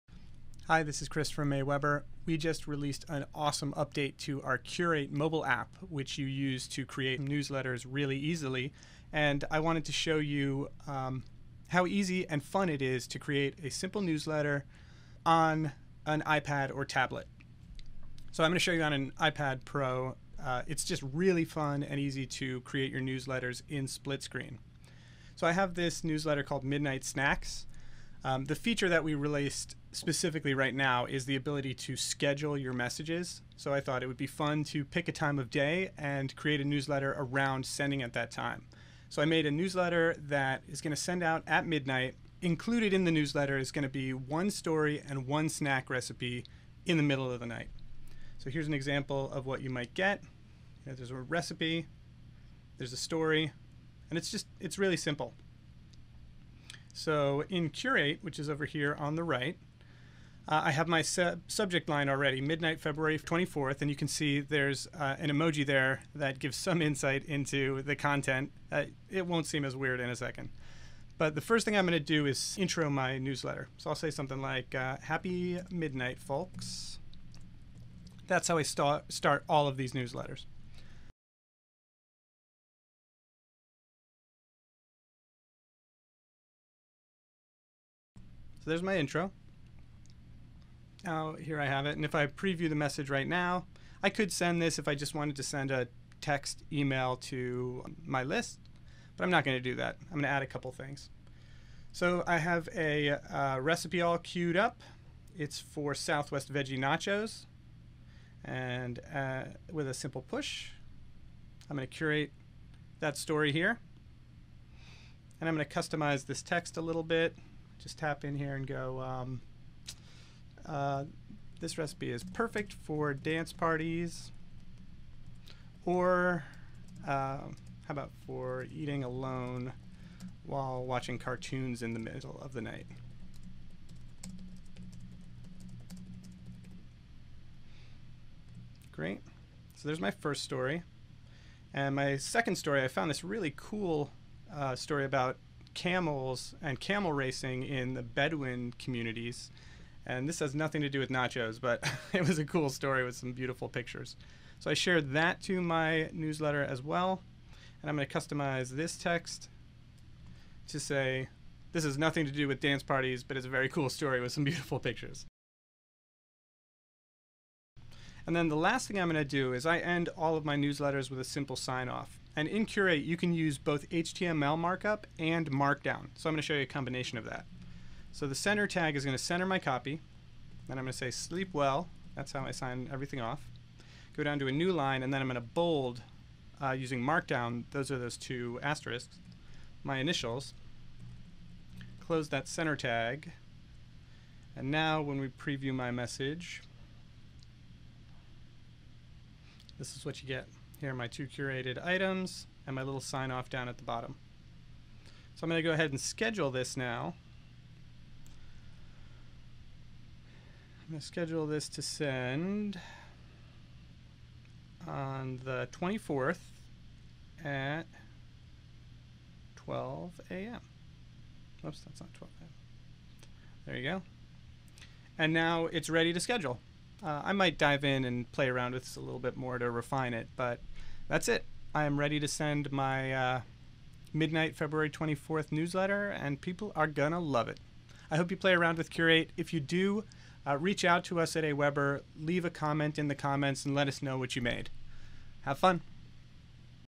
Creating an Email Newsletter on Your Tablet with Curate from AWeber
I wanted to show how fun, easy, and simply magical it feels to build newsletters in split screen on the iPad with Curate from AWeber.

Curate, from AWeber, makes it as easy as pie to create beautiful email newsletters by collecting and curating content for your audience. Get started curating and sending newsletters from your phone or tablet today!